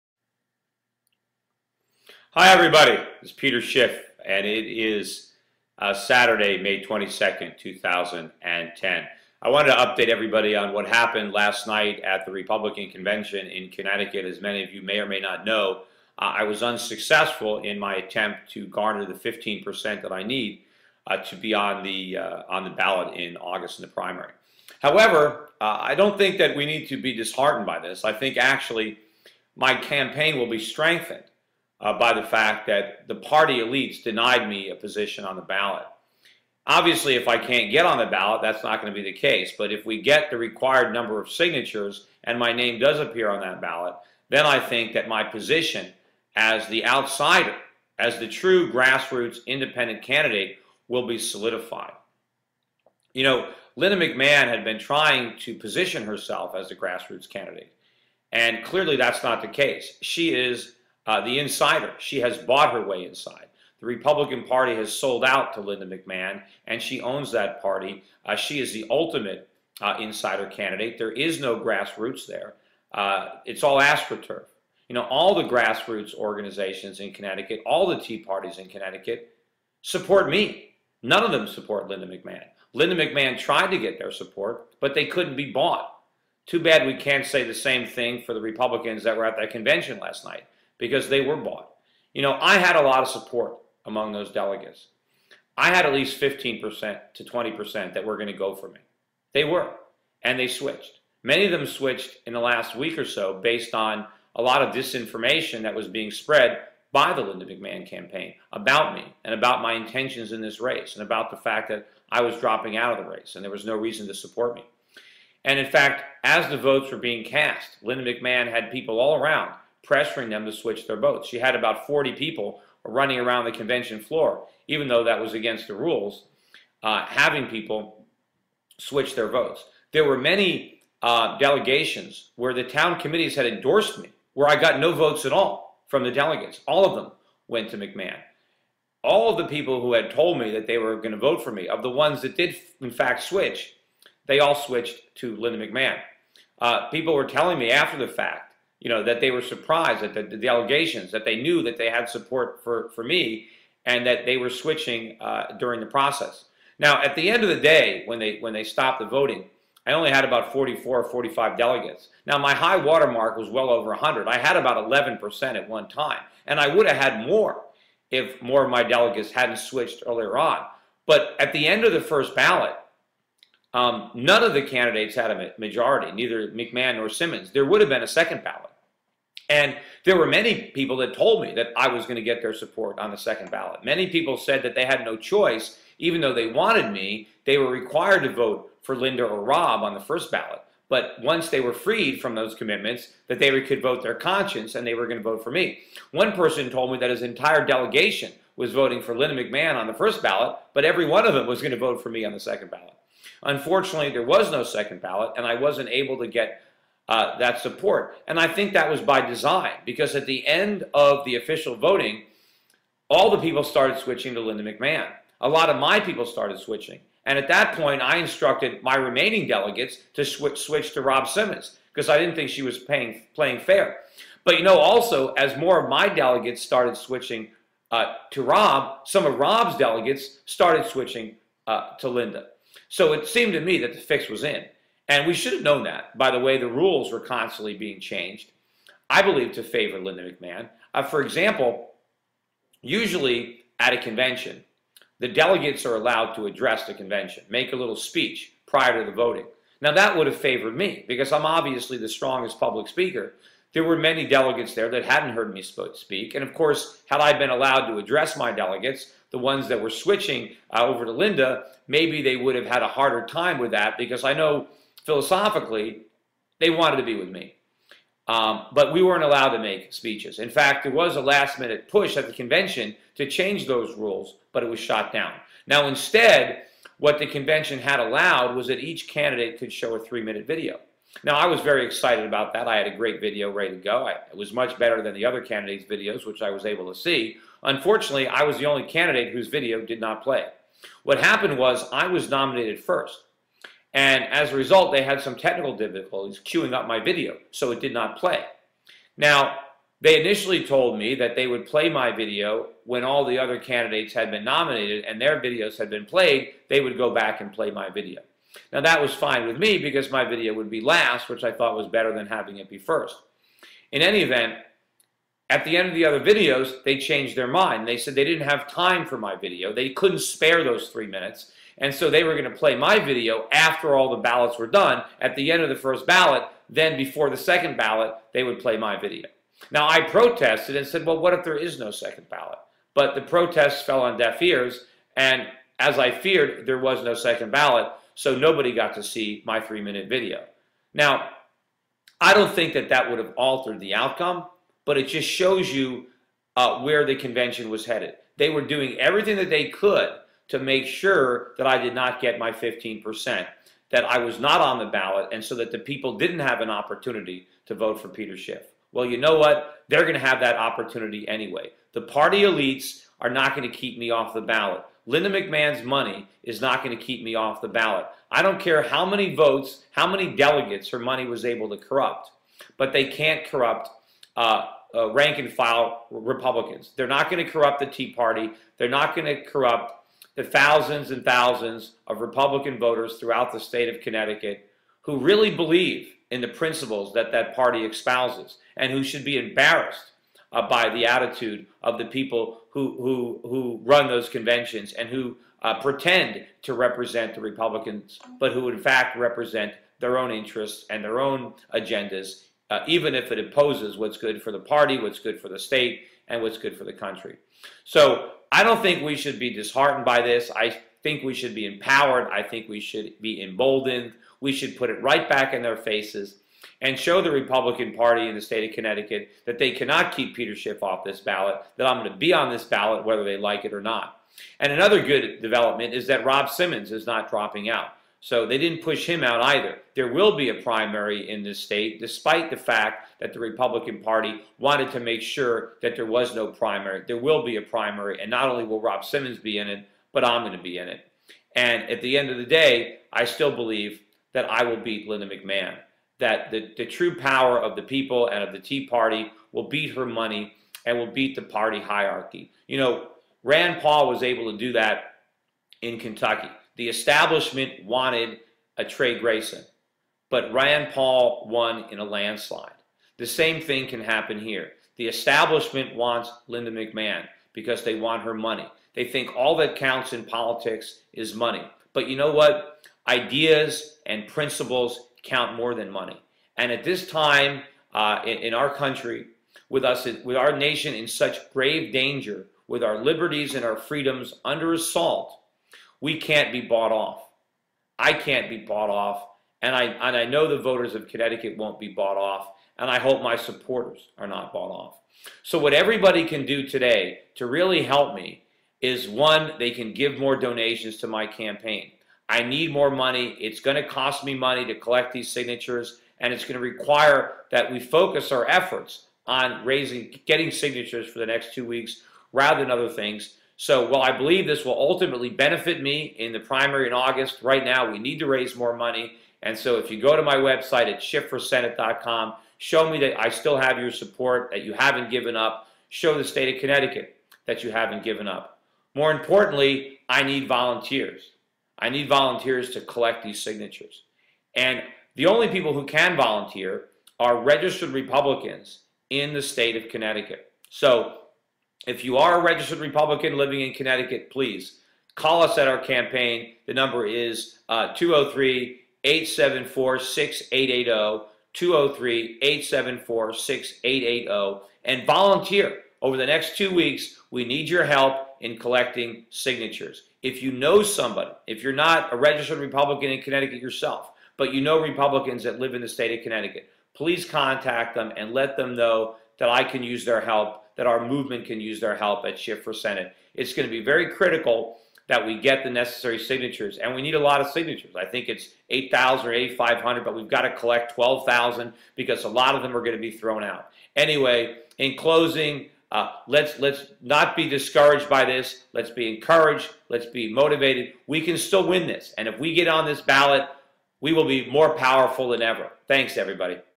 Schiff_for_Senate_-__Republican_convention_results.mp4
Linda McMahon is an insider. Peter Schiff has ALL the grassroots support in CT, including the Tea Party groups. Schiff is who we need in the Senate, not McMahon! Visit: SchiffforSenate.com and SchiffReport on youtube